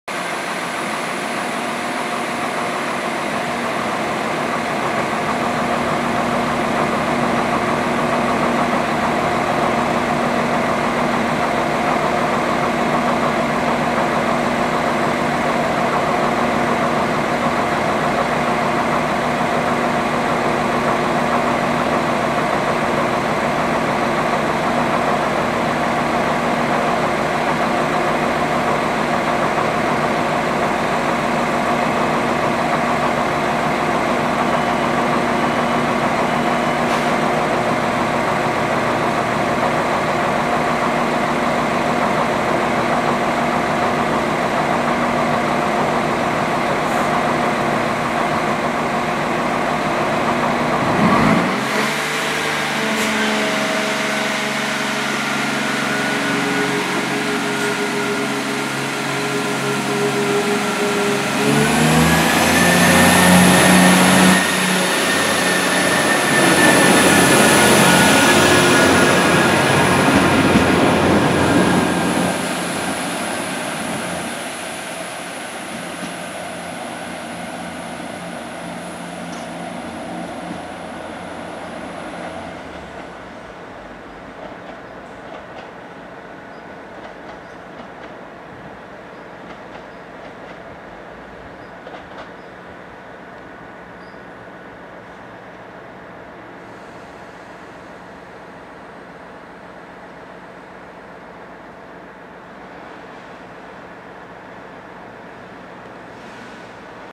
Irish Rail 22000 DMU 22053 departs & 22000 DMU 22023 departs Kildare
Date: 28 July 2014
Service: 10.15 Dublin Heuston to Waterford
Service: 10.22 Portlaoise to Dublin Heuston